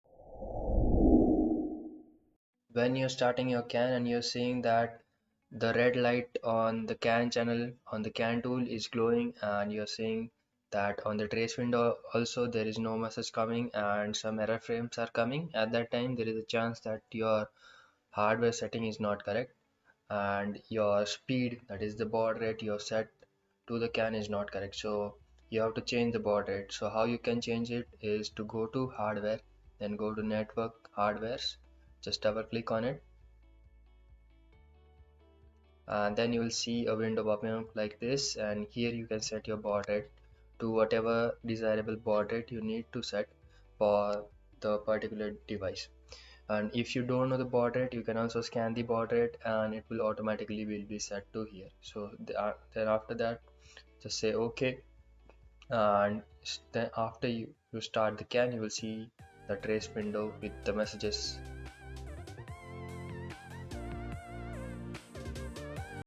How to set the baud rate in the vector CAN
This video shows the tutorial of how to set the baud rate in CANoe tool

Sometimes it happens when baud rate is not set correctly then we see CAN error in the trace window or the red LED on the channel of CAN tool, then the most probably baud is not correct. So by setting or by scanning the baud rate we can solve this error and get the messages in the trace window